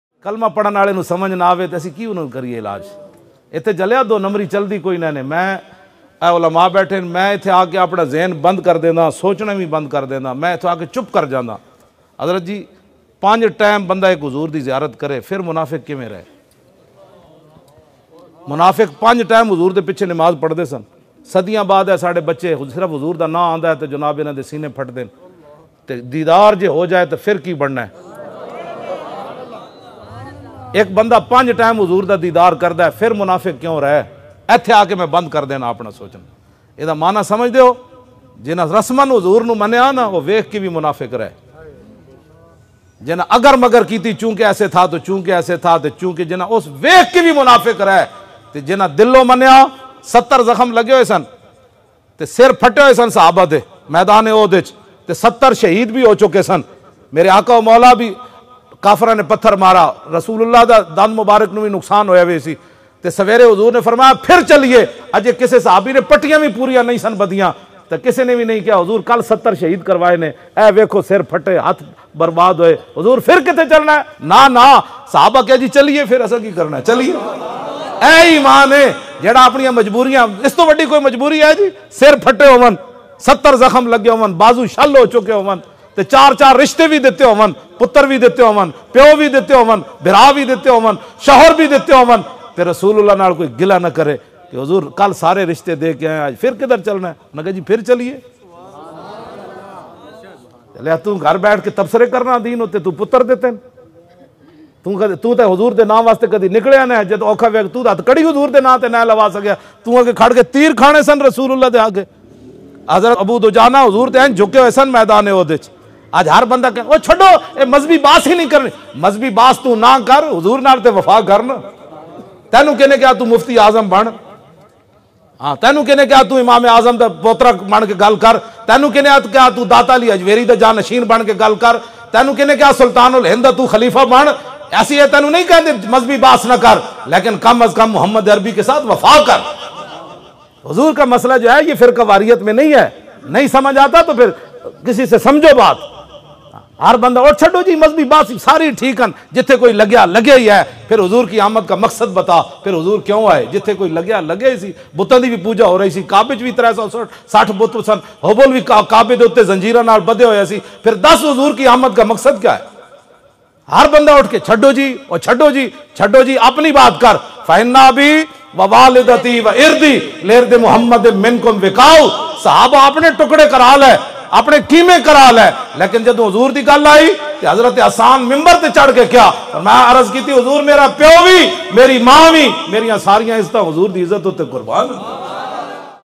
Allama Khadim Hussain Rizvi Official | RASOOL ALLAH ﷺ Ke Piche Namaz Parh Kar Bhi Munafiq Rahe
یہاں دونمبری چلتی کوئی نہیں

رسول اللہﷺ کے پیچھے نماز پڑھ کر بھی منافق رہے

For Latest updates, Speeches and Complete Bayanat's join TLP Official Social Media Network of Allama Khadim Hussain Rizvi.